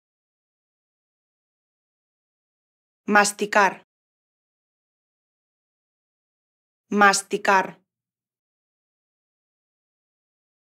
How to pronounce MASTICAR in European Spanish
This video show you how to pronounce MASTICAR in European Spanish. Speaker has an accent from Northern Spain. Collins Dictionary, the home of living language and pioneers of dictionary publishing